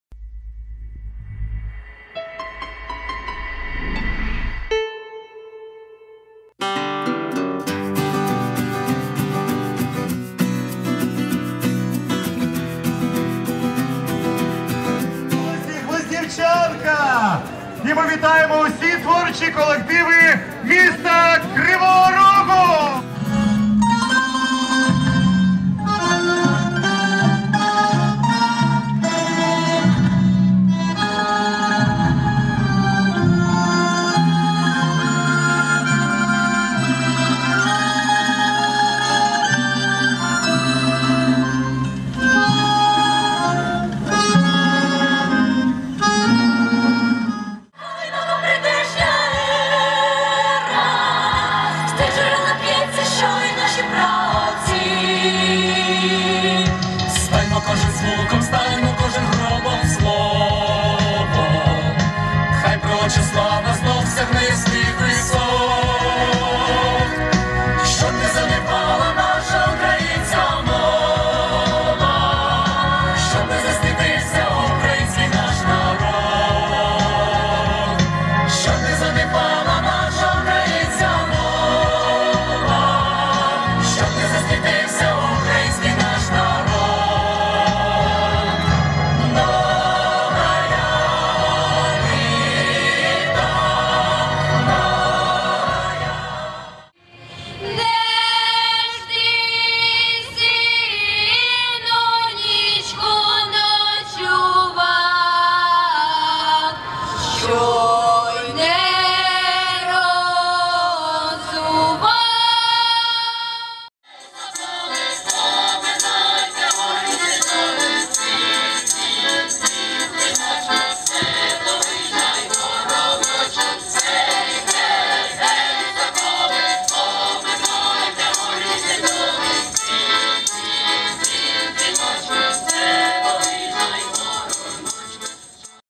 Культура Кривой Рог: Фестиваль козацкой песни | 1kr.ua
14 октября в криворожском парке Героев проводится III Всеукраинский фестиваль козацкой песни. Поучаствовать в мероприятии со всех городов Украины приехали тысячи музыкантов и вокалистов, среди них делегаты из Днепропетровской, Киевской, Николаевской, Запорожской и других областей.